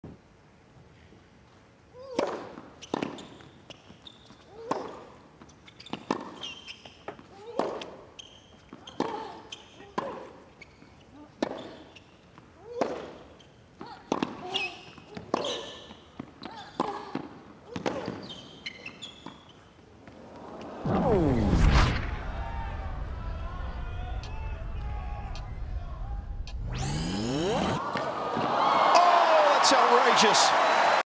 INCREDIBLE commitment from Coco Gauff! 💪
Coco Gauff pulls of an amazing get to win the point against Caroline Dolehide in the second round of the Australian Open 2024.